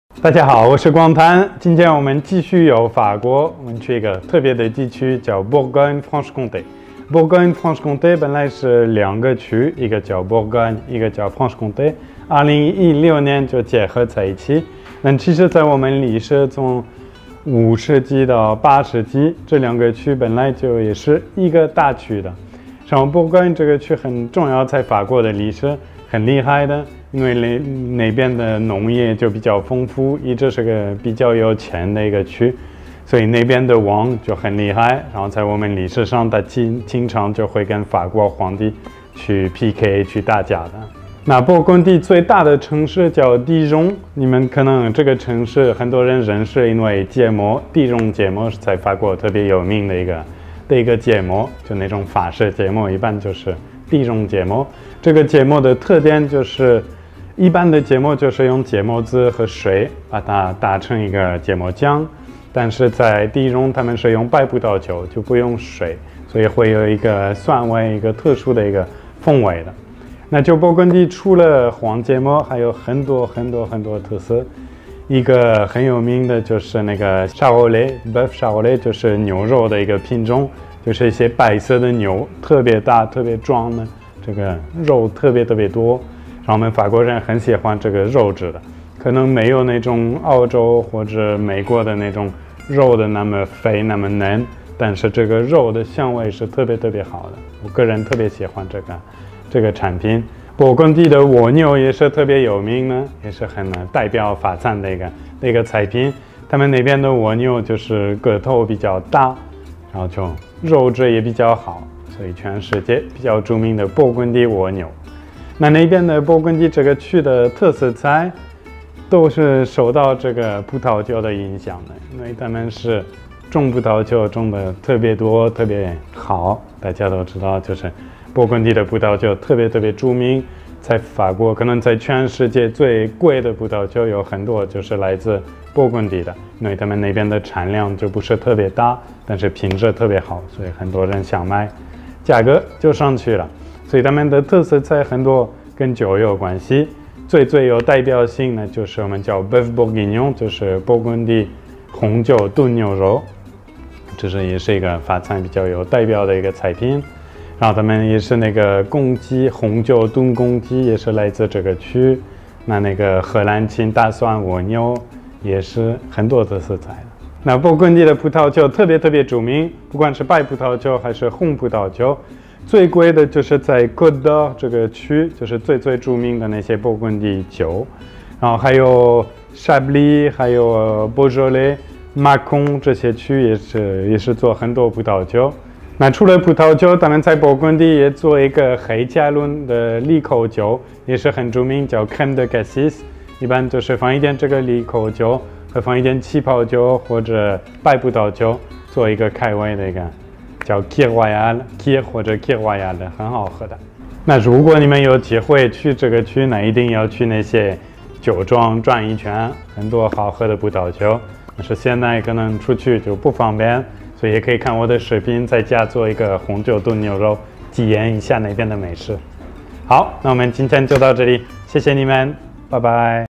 Bourgogne-Franche-Comté - France Food Tours 跟着广坦游法国 - 勃艮第-弗朗什孔泰大区篇
勃艮第不止有红酒，还有蜗牛和芥末。本周带领大家来到法国东部的勃艮第-弗朗什-孔泰大区，一起踏上美食探索之旅！